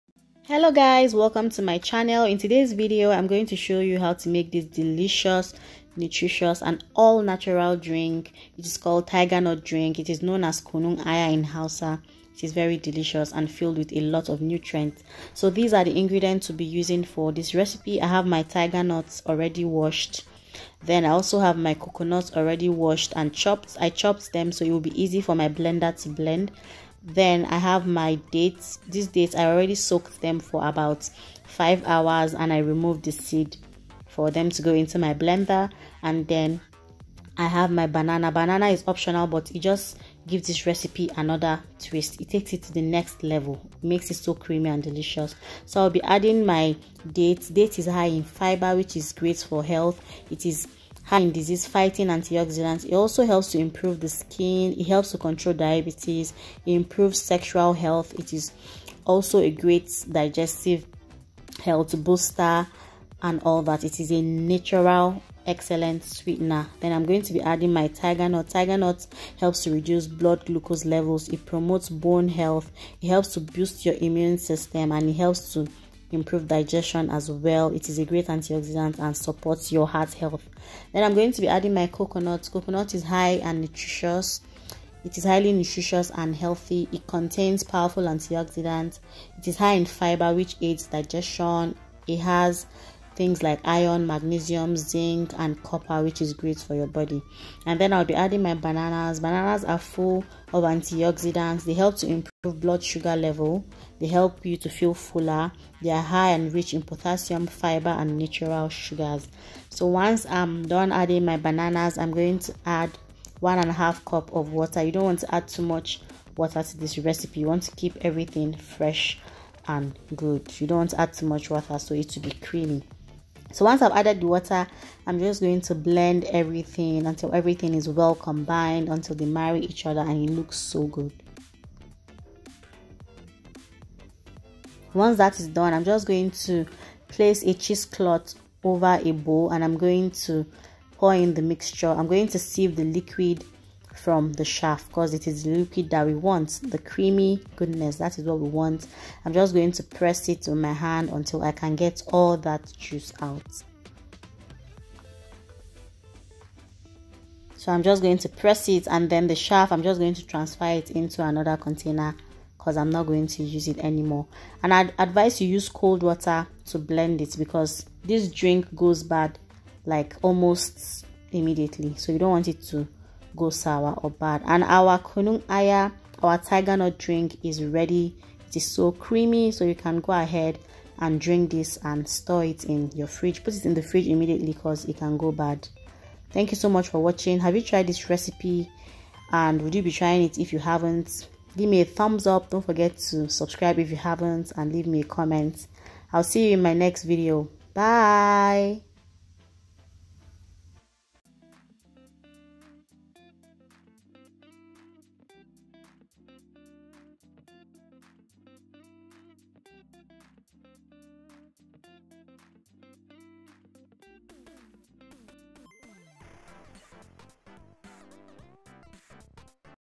Nigerian Aphrodisiac: Tigernuts, Coconuts, Dates & Banana Drink
Tiger Nut drink is very nutritious,healthy and super delicious.
There is one tip for making this drink so it doesn't go bad quickly, blend it cold water to give it a good temperature.
Ingredients
* Dates (soaked with seeds removed)
* Tigernut
* Coconut
*Banana

Benefits of Dates:
* Dates are high in fiber which is great for health.
* It is an excellent natural sweetener.
* Dates improves sexual health.
* It improves digestive health.
* It helps to control diabetes.
* Dates helps to improve skin.

Benefits of Tigernut :
* Tigernut helps to reduce blood glucose  levels.
* It promotes bone health.
* Tigernut helps to boost immunity.
* It is a great antioxidant.
* Supports heart health.

Benefits of Coconut:
* Coconut is highly  nutritious and healthy.
* It contains powerful antioxidants.
* Coconut has high fiber which helps to aid digestion.
* It is rich in iron, magnesium, zinc,copper and selenium.
* It has anti aging properties.
* Coconut supports bone and teeth health.

Benefits of Banana :
* Bananas are filled with antioxidants.
* Bananas  help improve  blood sugar level.
* Banana is calorie dense so it makes you fill fuller.
* Bananas are high in potassium, fiber and natural sugars.